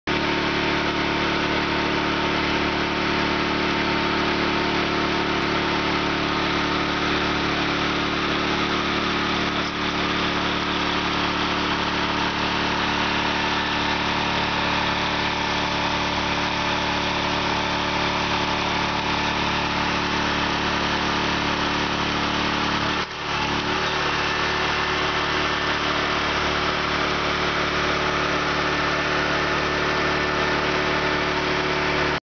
MLTUS Pull-Behind Magnet Sweeper Demo
Demonstration of the pull-behind magnet sweeper from MLTUS. A fork mounted version of this device is also available. Contact (877) 50-MLTUS for more information.